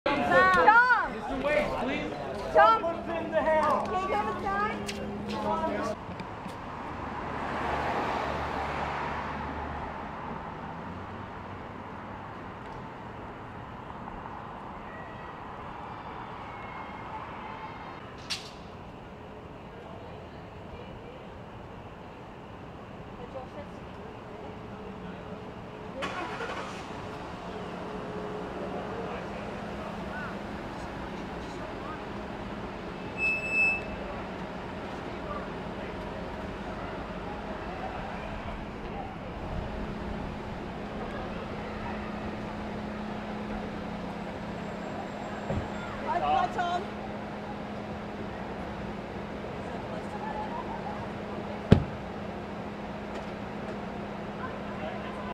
Tom Waits and Kathleen Brennan depart the Seven Psychopat...
Tom Waits and Kathleen Brennan depart the Seven Psychopaths After Party in Westwood, 10/01/12